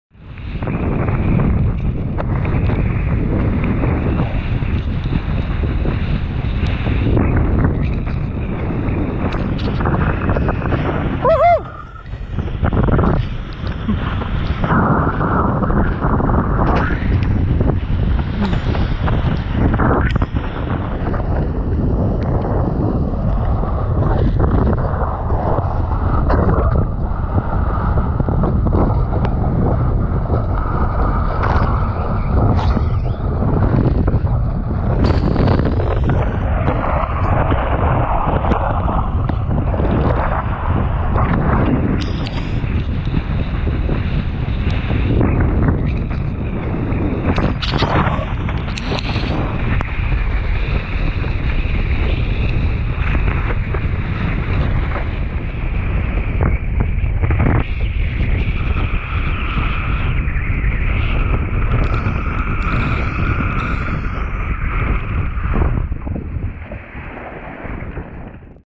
Savudrija 04.02.2019 - kitesurf with "new" board :)
Testing out my "new" board... unfortunately, forgot to adjust the foot straps before jumping into the sea :(